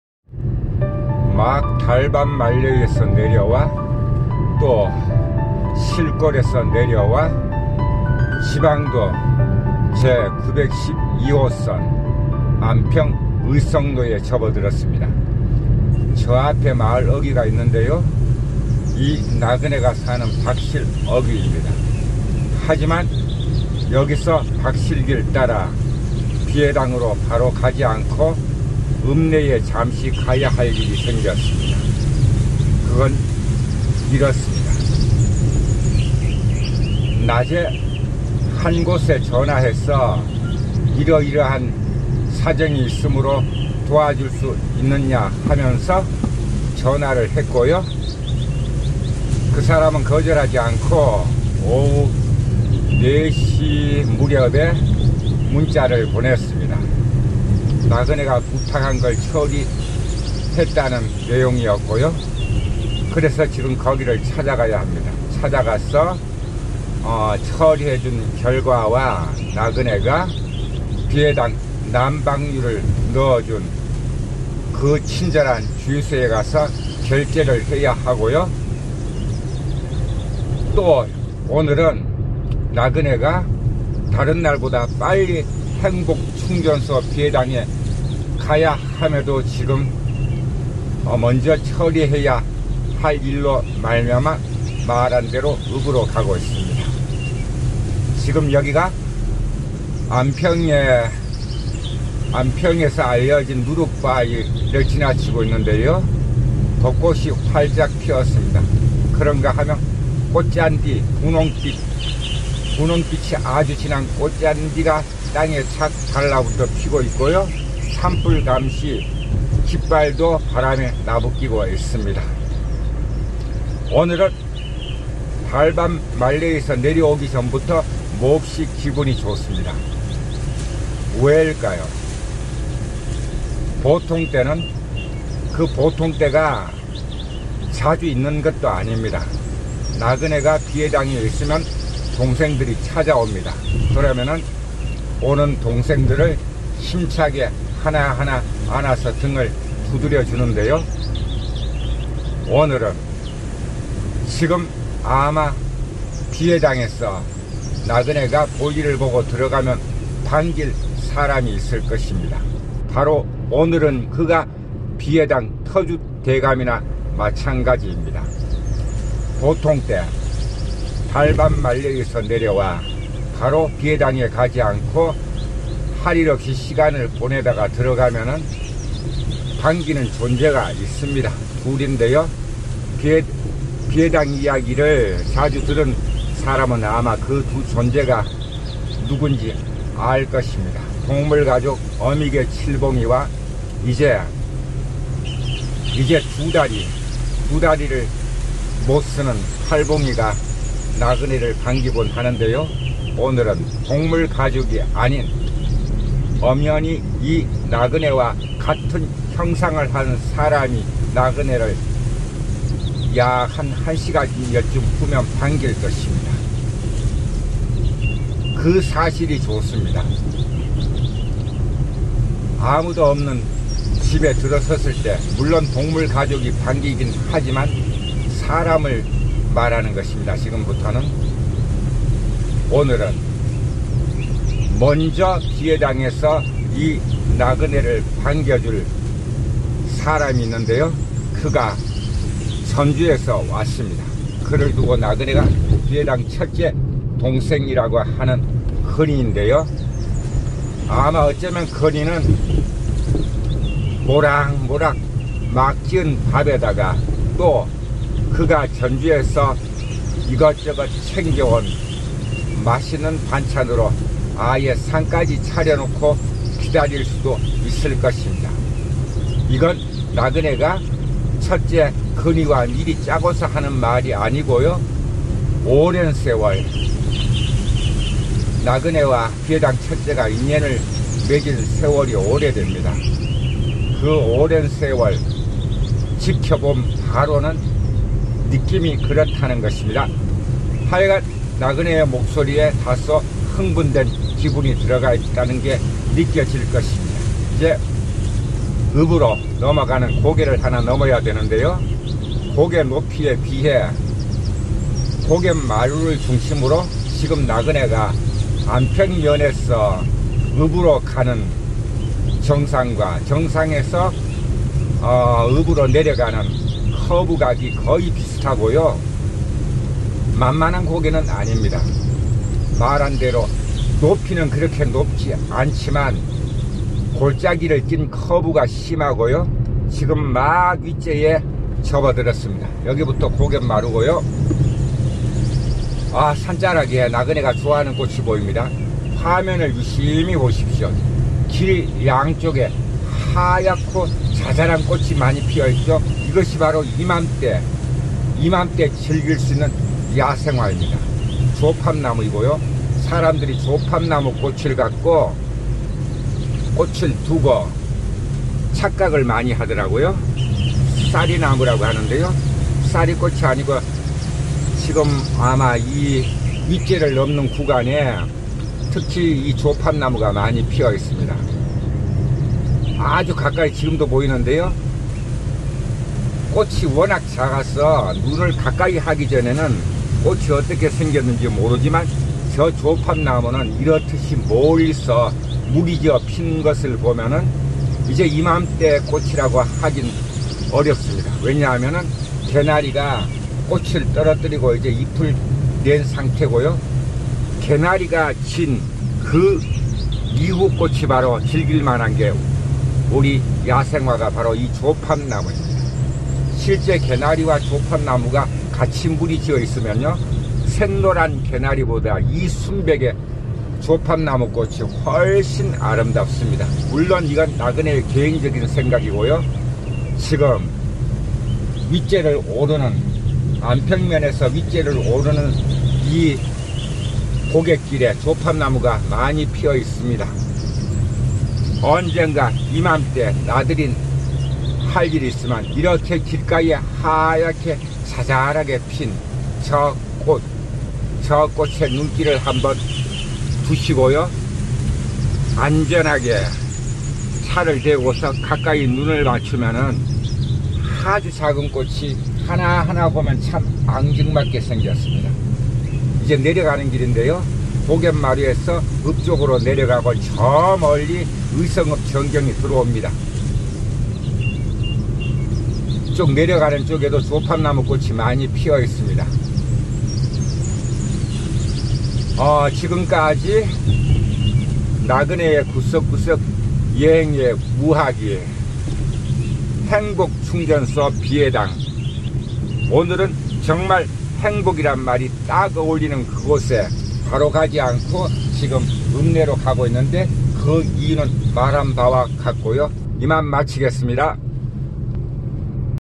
나그네의 길따라 마음따라, 오늘(210409) 주객이 바뀐 비해당
나그네의 길따라 마음따라
오늘(210409) 주객이 바뀐 비해당
지방도 제912호선 안평의성로에서
이맘때 우리 산야의 들꽃 야생화 이야기
소리로 듣는 자유인 생각